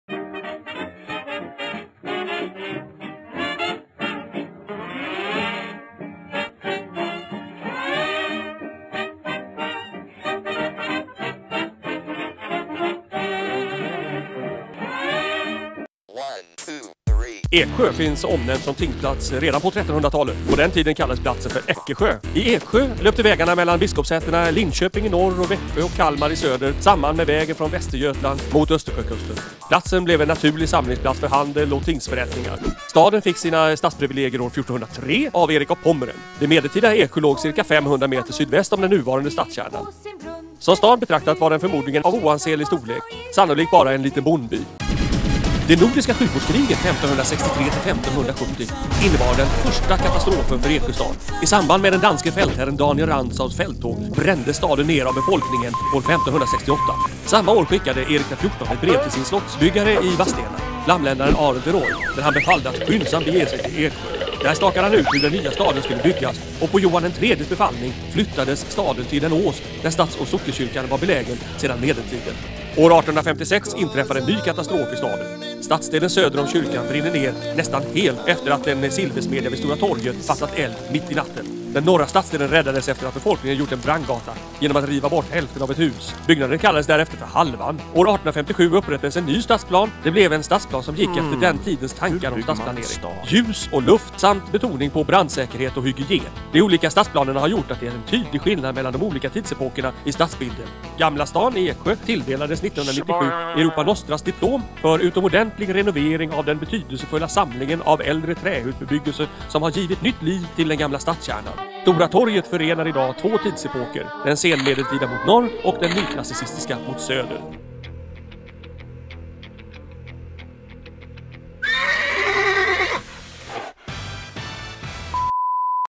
Eksjöhistoria
Kort sammanfattning av Eksjös historia. En del av kommande dokumentärfilmen om Eksjö.
Voiceover Göran Modig